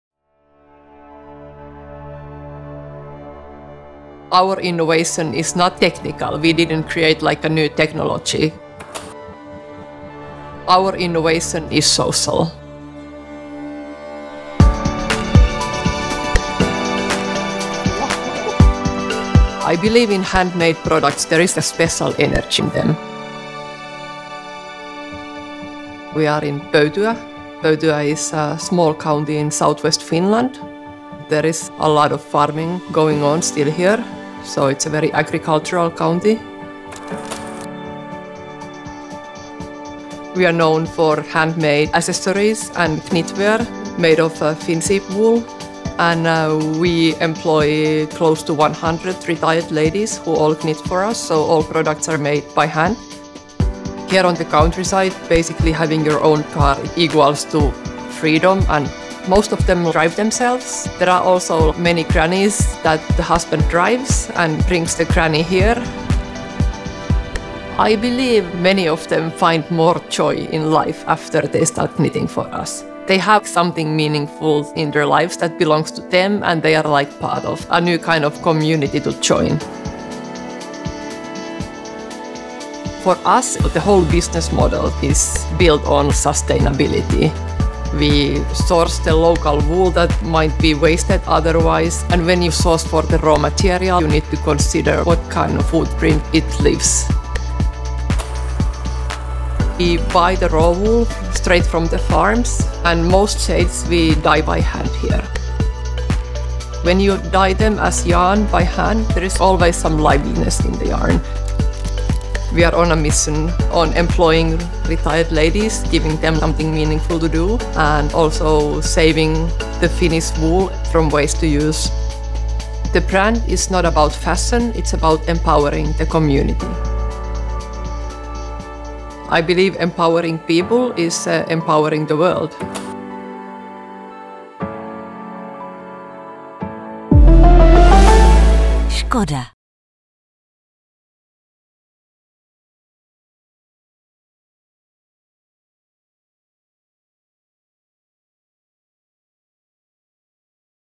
Explorers of Sustainability: Close-knit community, eco-friendly knitwear
Eco-friendly innovation doesn’t always have to involve creating a new technology. Sustainability can be social, too – as Anna Rauhansuu shows us.​
Her business, Myssyfarmi, sources raw wool from waste that would usually be tossed aside, to create handmade knitwear and accessories.​
They employ almost 100 retired ladies from the surrounding agricultural countryside, creating a close-knit community who do  meaningful work long into their later years. Doing something you love every day? Certainly a sustainable way of working.​
And, in this rural setting, the cars they drive give them the freedom to do it. As Anna says, “Empowering people is empowering the world.”​